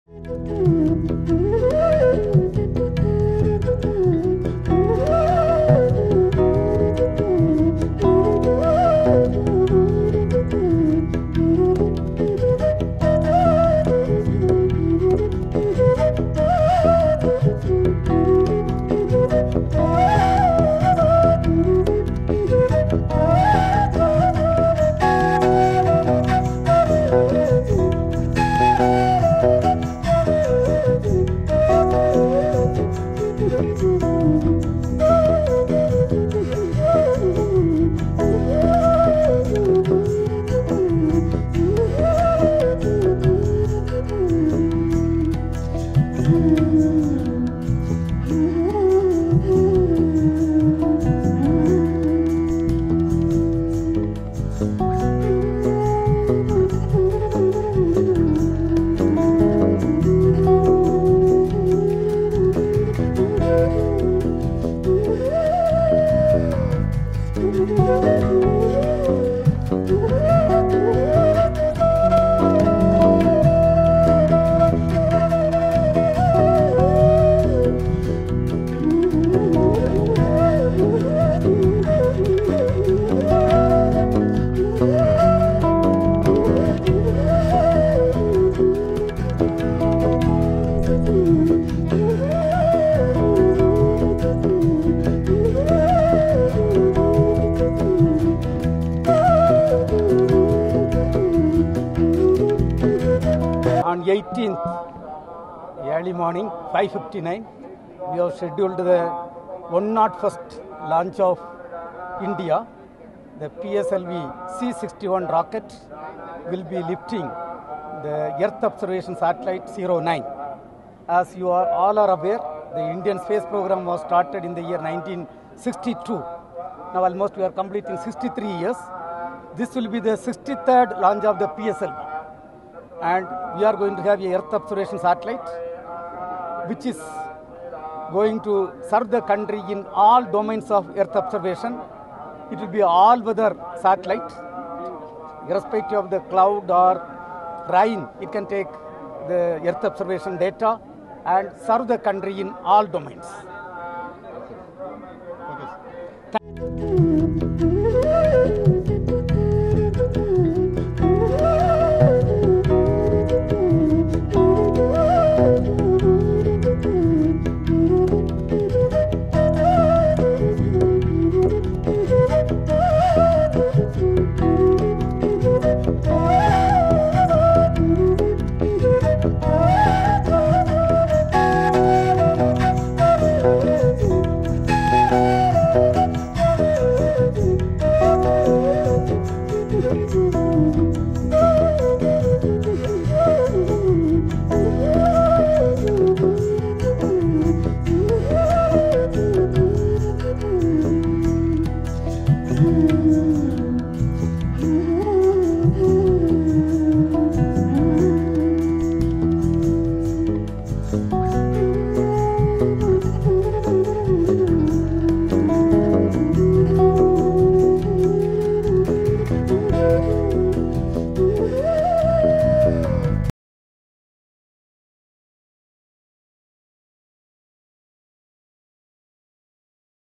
ISRO Team at Tirumala Temple for Successful of RISAT-18 Satellite | PSLV-C61 | Samayam Telugu
ISRO officials to seek blessings at the Sri Venkateswara Temple in Tirumala before significant satellite launches. This tradition reflects the deep-rooted cultural and spiritual beliefs in India.
ISRO is preparing for the launch of its 101st satellite, the Earth Observation Satellite RISAT-18, aboard the Polar Satellite Launch Vehicle (PSLV-C61) on May 18, 2025. The launch is scheduled to take place from the Satish Dhawan Space Centre in Sriharikota at 6:59 AM IST.